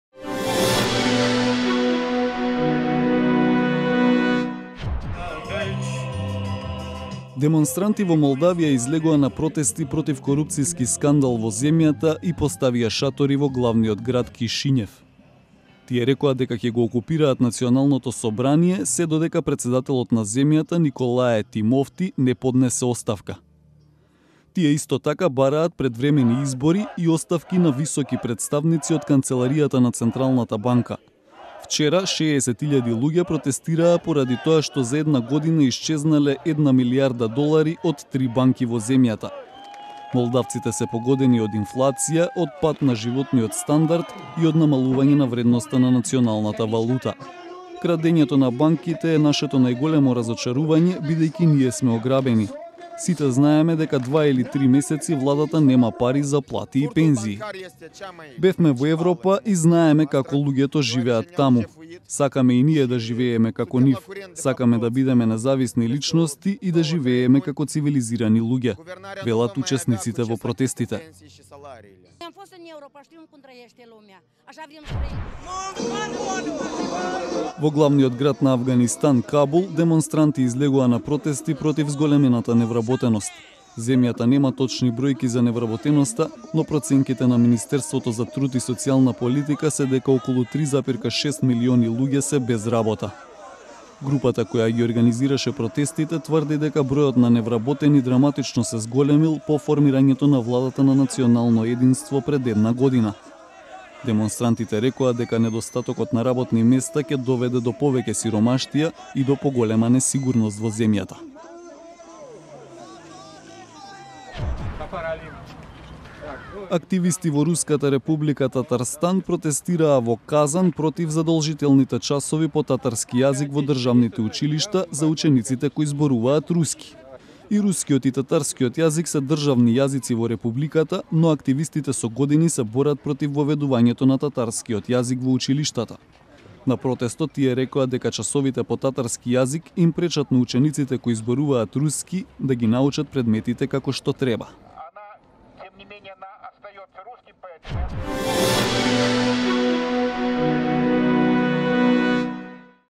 07.09.2015 Протести во Кишињев, Кабул и Казан
ТВ вести од регионот и светот на Радио Слободна Европа.